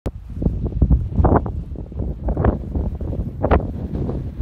函館山の漁火公園（2021年5月）
北海道函館市にある函館山の漁火公園の景観です。
函館山山頂屋上展望台の同じ景観が眺められます。